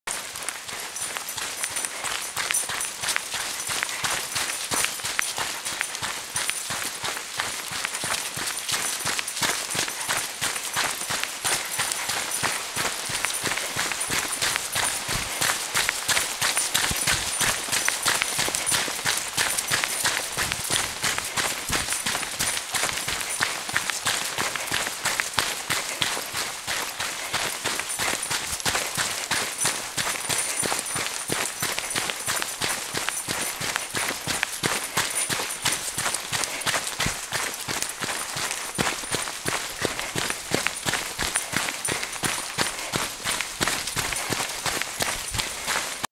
Il mio personal trainer!:)))
con Baloo a correre nei boschi!